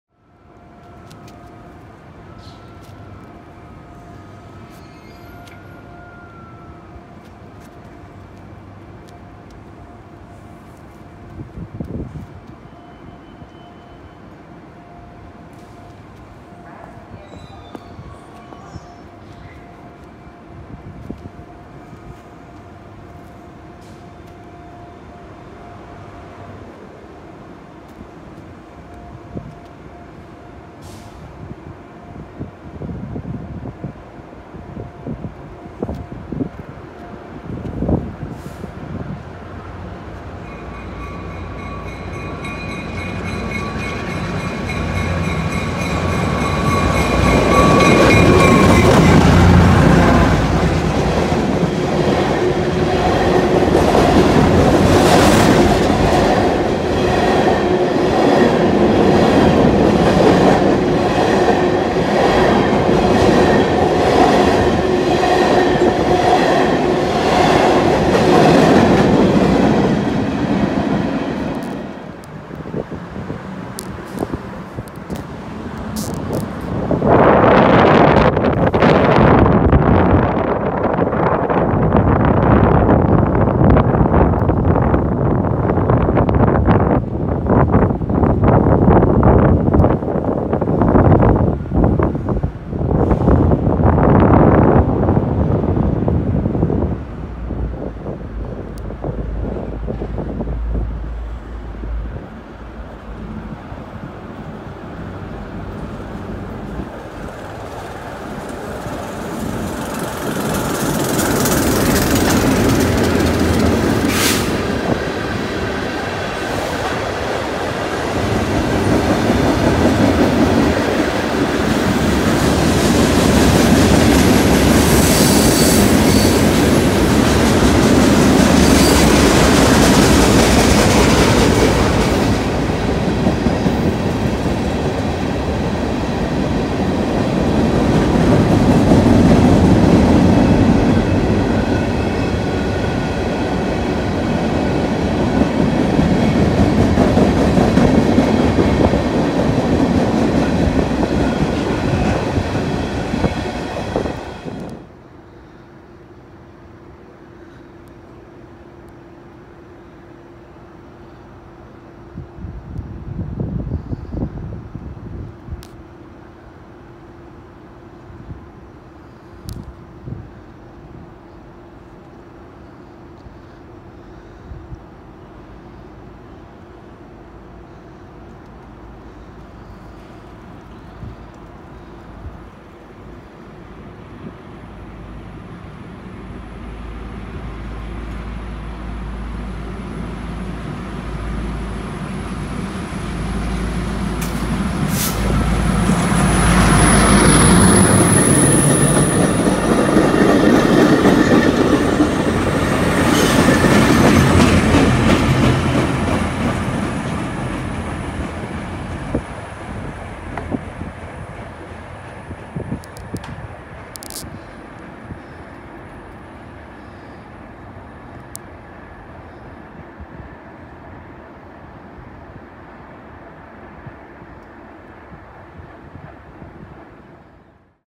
LIRR laser train and NYAR RS51 and RS80 at Mineola
Taken on Tuesday November 23rd, 2021. Enjoy!